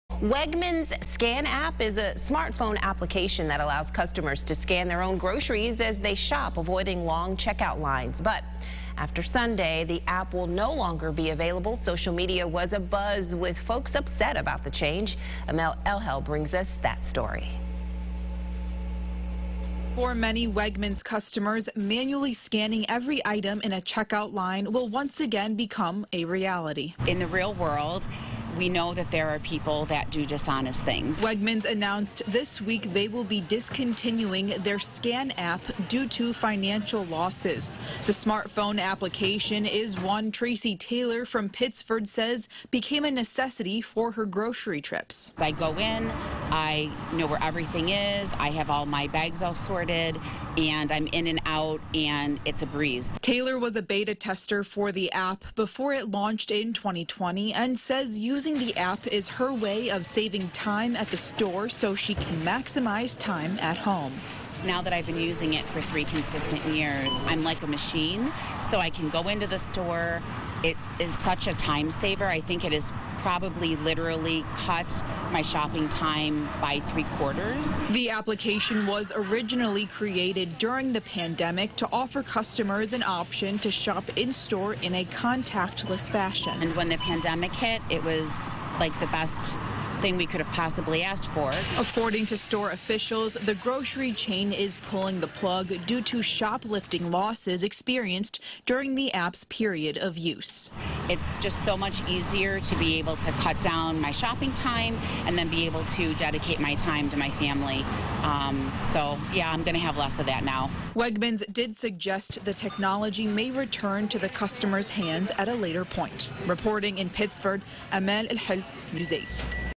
Wegmans shutting down SCAN App, points to shoplifting losses
The SCAN App will be available up until Sunday, September 18. The application was originally created during the COVID pandemic to offer customers an option to shop in-store in a contactless fashion.